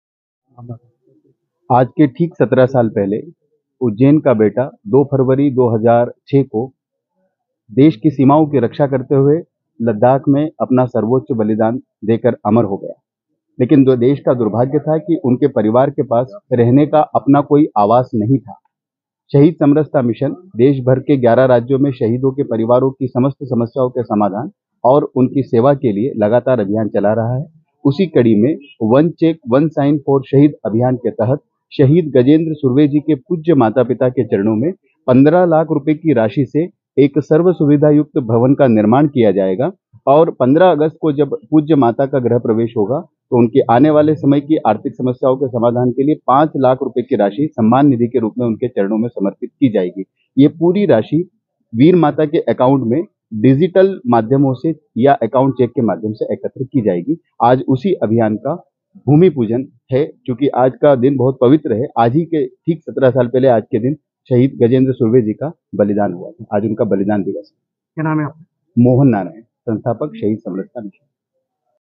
भवन निर्माण, वीरभूमि पूजन, उज्जैन | Mohan Narayan | Ujjain
भवन निर्माण, वीरभूमि पूजन, उज्ज्जैन | Ekatma Bharat

17 वर्षों का इंतजार खत्म, शहीद गजेंद्र राव सुर्वे की पूज्य माता–पिता को कृतज्ञतापूर्वक आगामी 15 अगस्त को 15 लाख की राशि का "सम्मान भवन" और 5 लाख "सम्मान निधि" के साथ उनका गृह प्रवेश करवायेगा शहीद समरसता मिशन.
"शहीद का परिवार, हमारा परिवार